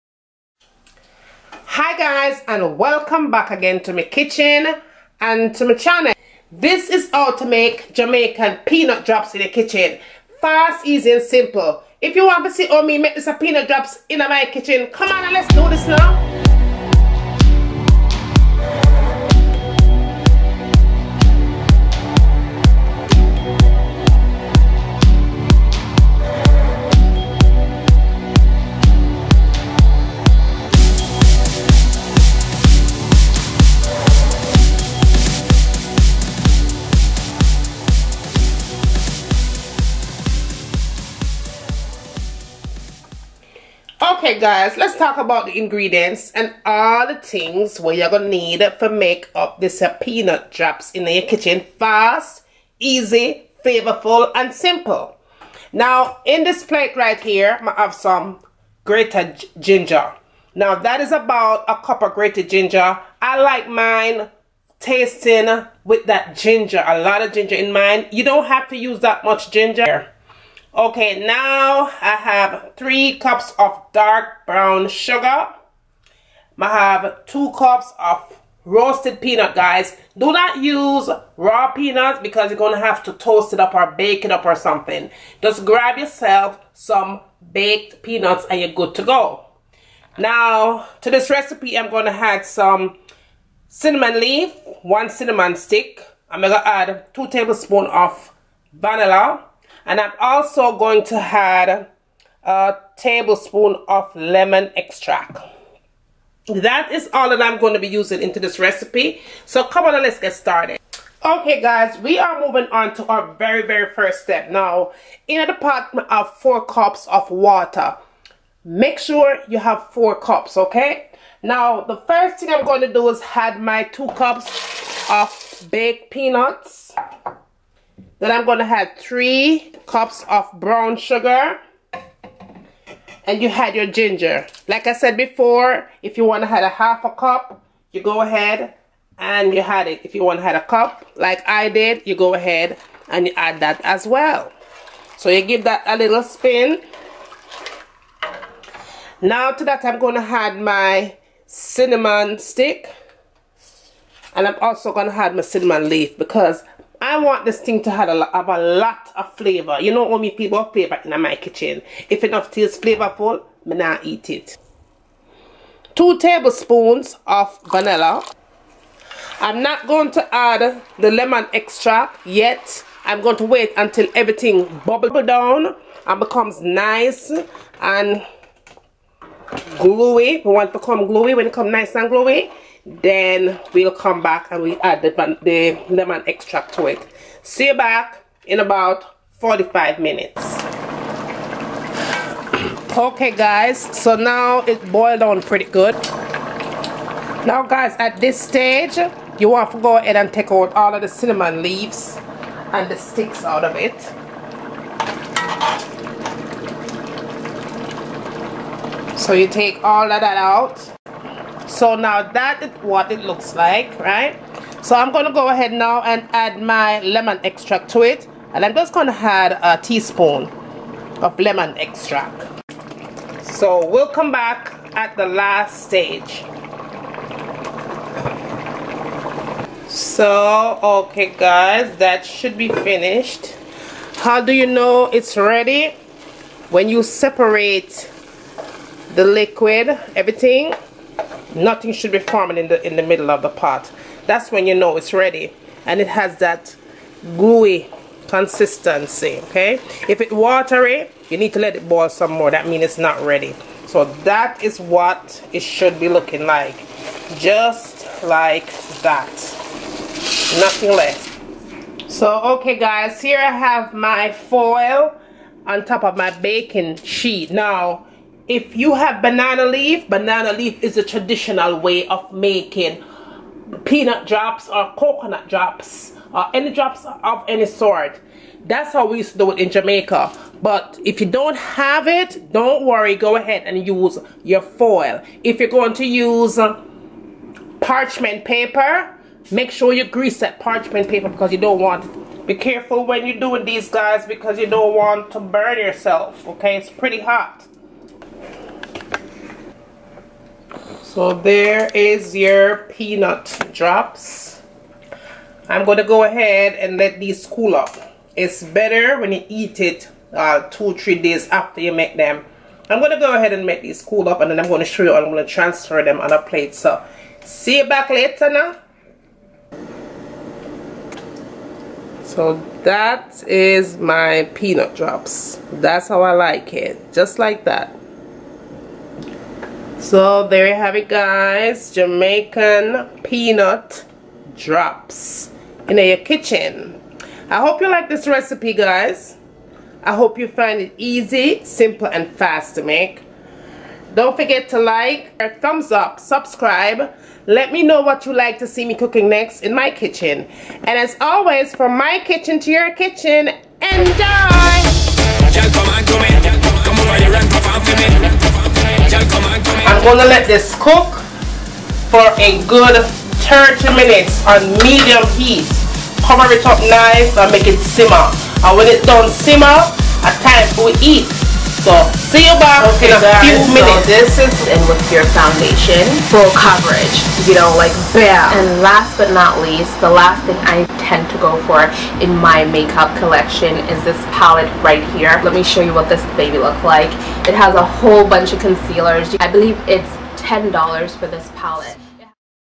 HOW TO MAKE JAMAICAN PEANUT DROPS RECIPE 2017
Hey Everyone,

Here is a JAMAICAN PEANUT DROPS CAKE RECIPE YOU GUYS ASKED FOR...  I hope that you found this easy to follow.

THINGS USE FOR THIS RECIPE
12 CUPS BAKED PEANUTS
3 CUPS BROWN SUGAR
1/2 CUP GINGER
1 TEA SPOON LEMON OR ALMOND EXTRACT
CINNAMON LEAVES
CINAMON STICK
TEA SPOON VANILLA

MORROW GA
30260

{ MORE HOW TO -JAMAICAN FOOD}